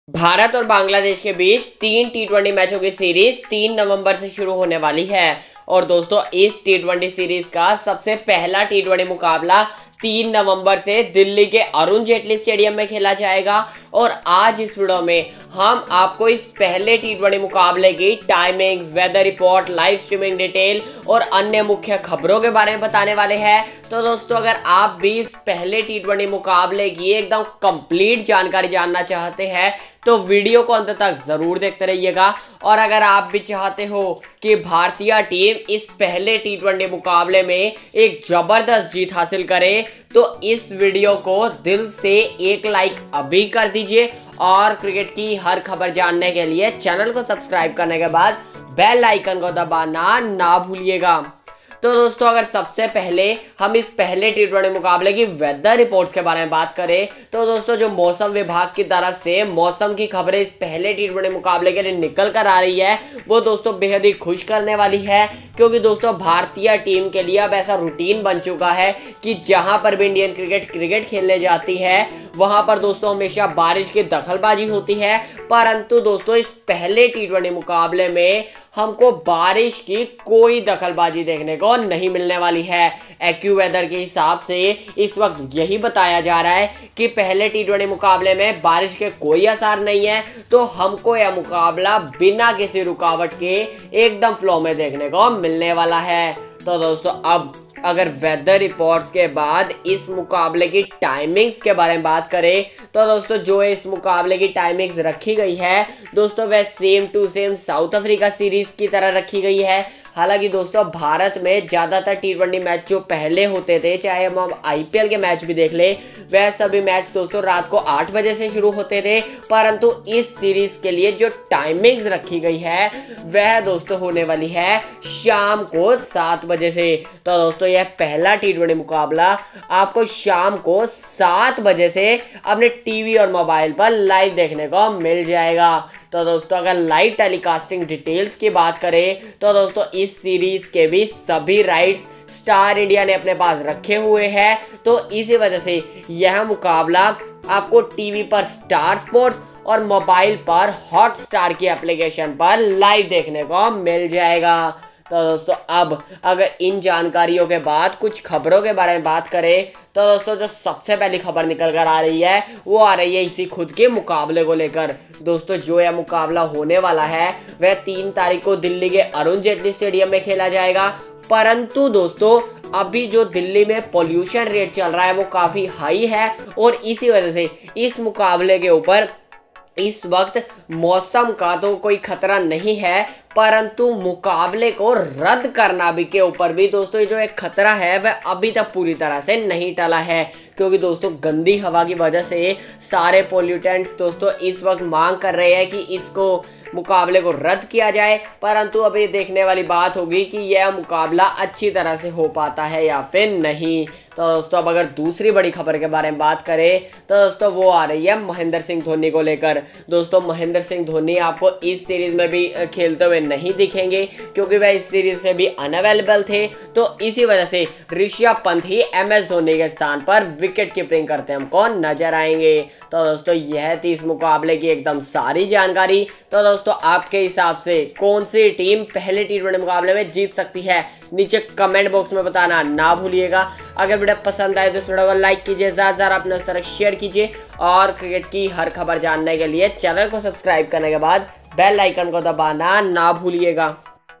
IND vs BAN 1st T20 - Rain Report & 2 Bad News
🎥Video BY Cricket is all